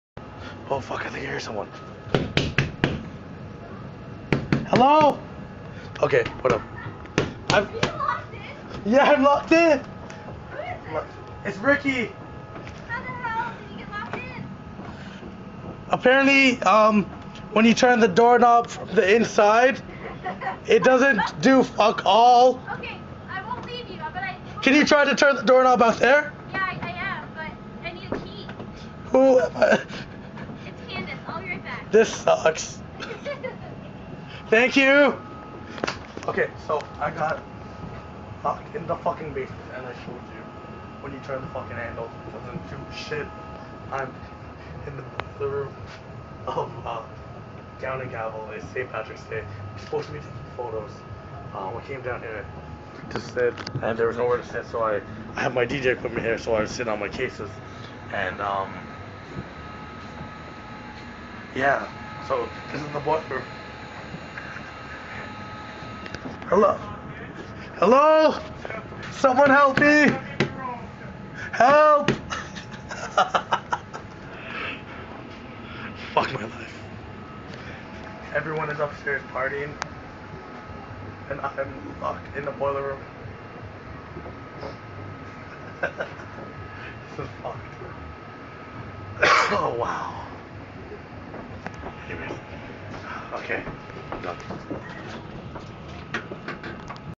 St. Patricks day.. stuck in the basement of Gown and Gavel!!!!
I'm stuck in a basement sitting on a tricycle.. girl gettin on my nerves! I'm going out of my mind I thought she was fine... don't know if her body is hers!!!

hahaha... Somehow I got locked in the furnace room in the basement at the gown and gavel with like 2000 people above me partying like rock stars... I was just starting the video when all of a sudden I heard Candice to teh rescue.. I haven't seen the video yet.. I'll see it for the first time on youtube!! hahaha